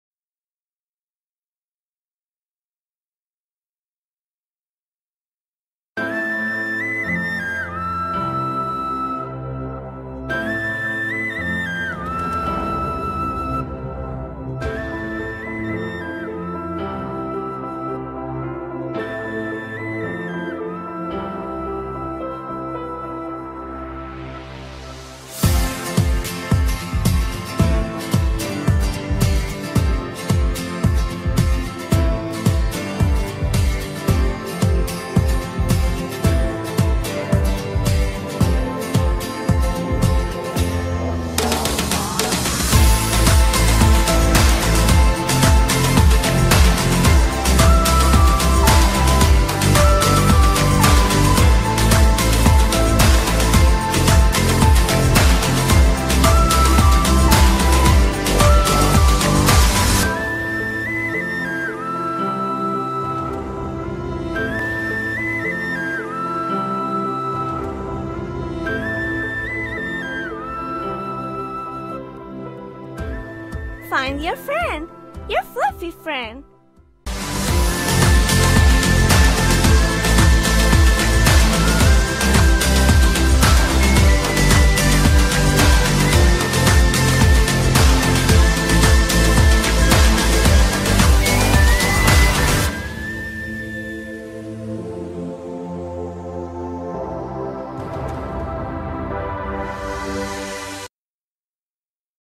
PAP - Fluffy
Prova de Aptidão Profissional realizada pela aluna Maria Gonçalves do Curso Técnico de Desenho 3D